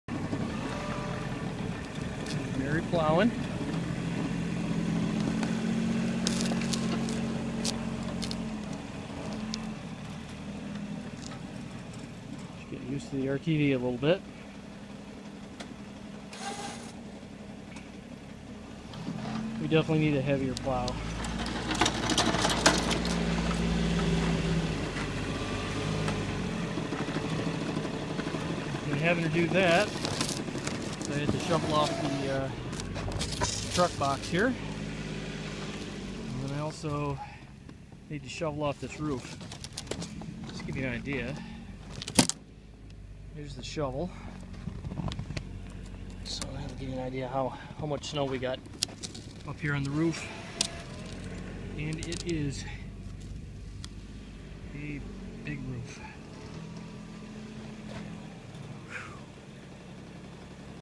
Marys first time plowing with the RTV900.
She actually did pretty good. The plow is for a quad, so it's under rated by about 2000lbs. That makes it trip a lot more than it should, but that's better than folding.
Next year, we'll get a real one, but this did surprisingly well.